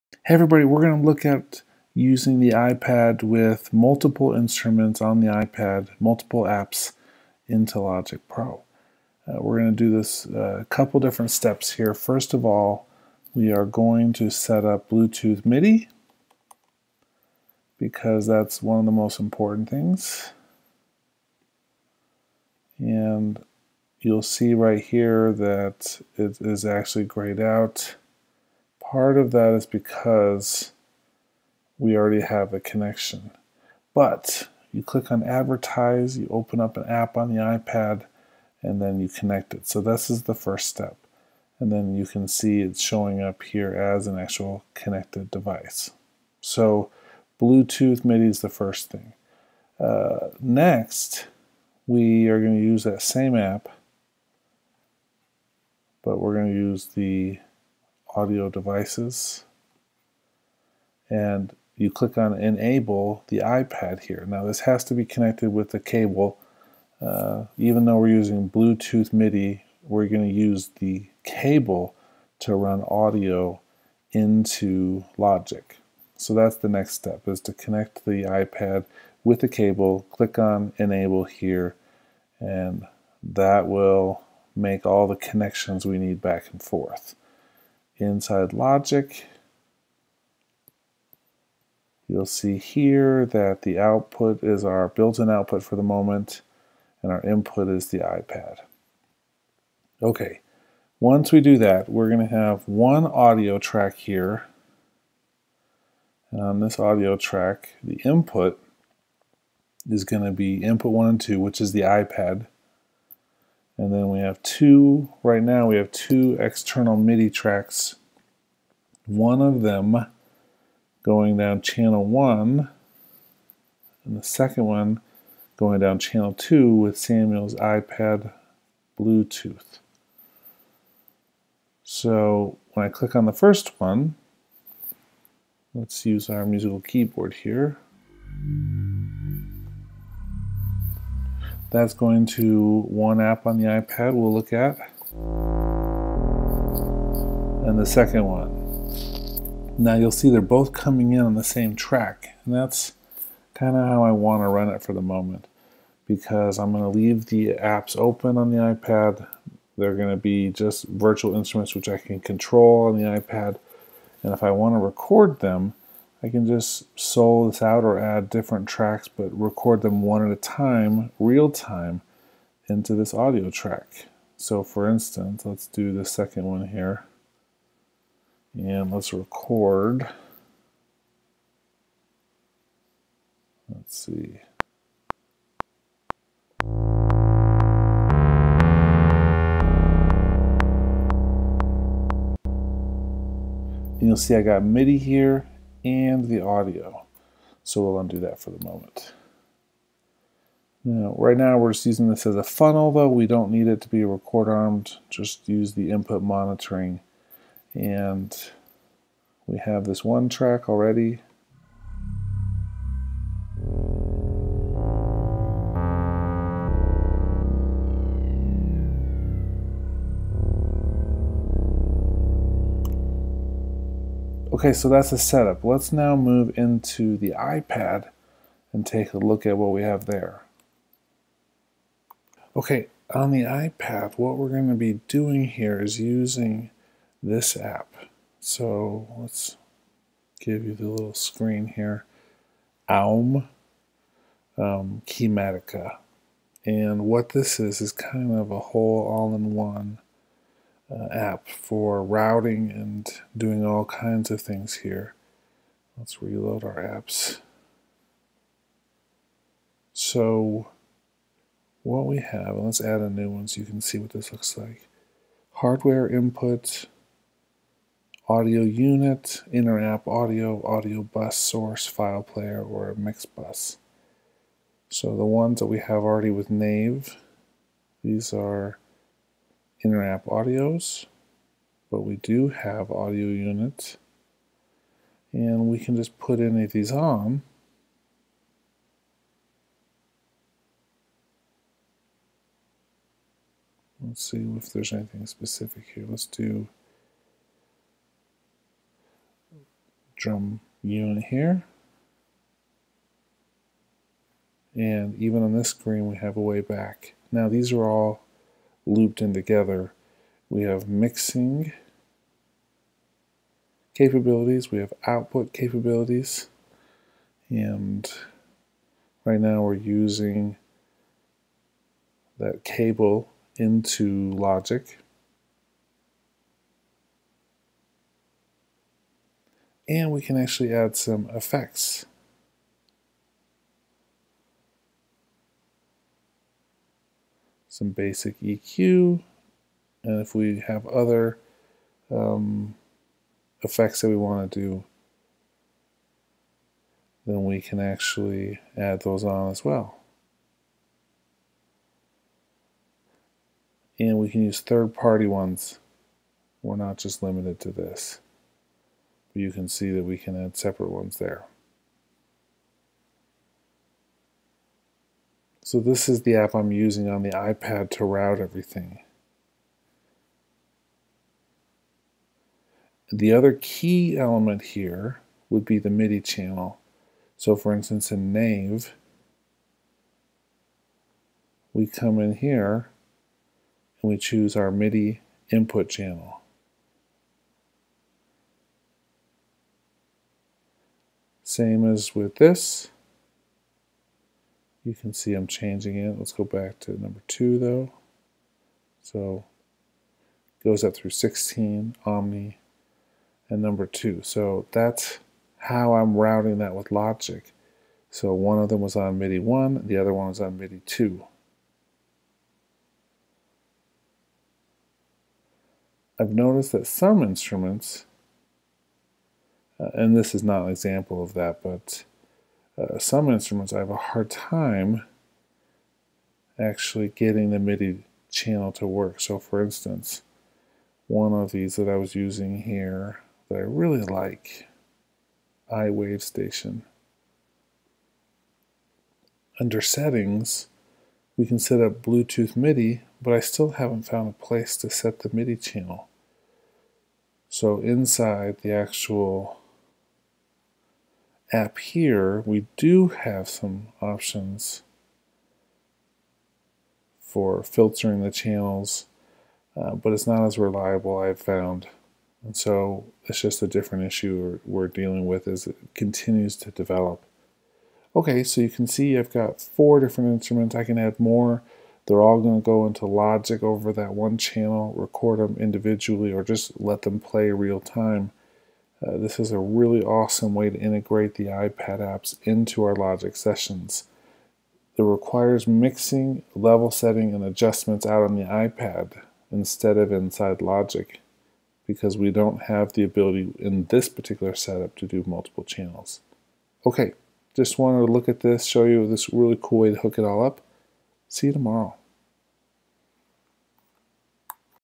Integrating Multiple iOS Synths into Logic Pro X
This video uses Bluetooth MIDI, the iPad as an audio input, and the AUM app from Kymatica to create an awesome extension to the Logic toolkit.